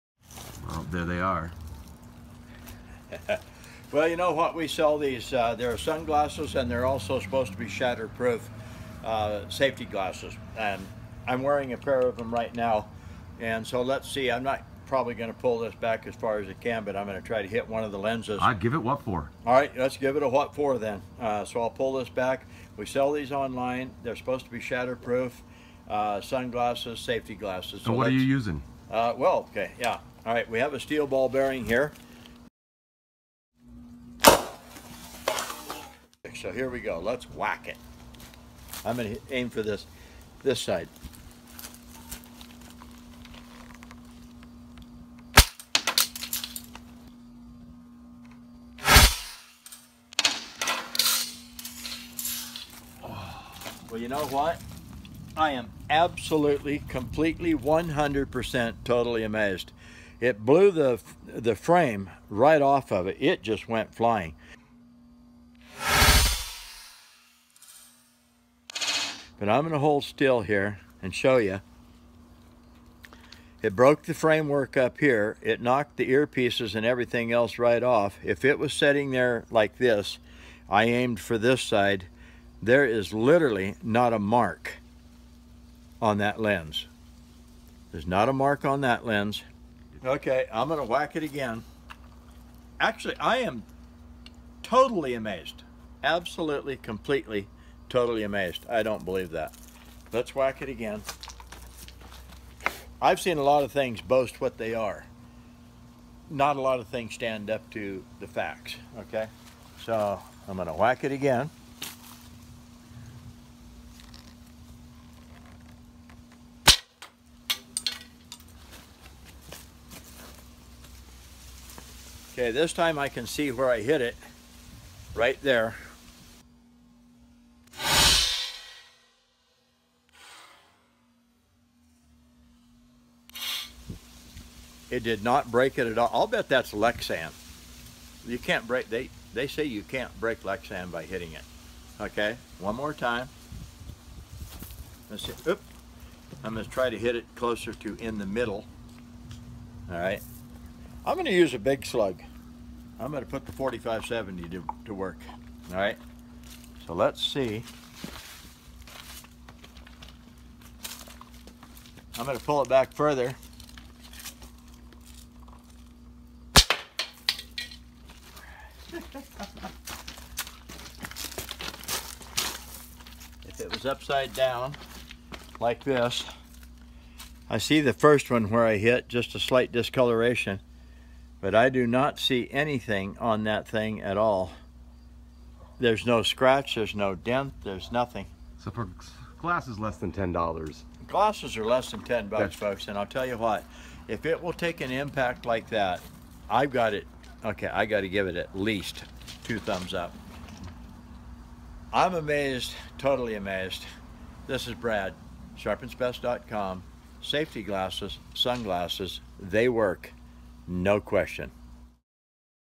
ShatterProof Eyeglasses vs. Steel Ball Bearing Slingshot
WHAT!?  Wow! These Shatterproof Glasses stand a Real impact test. -
 Less than $10 at www. SharpensBest.com

"The World's Best Knife Sharpeners and Survival Tools"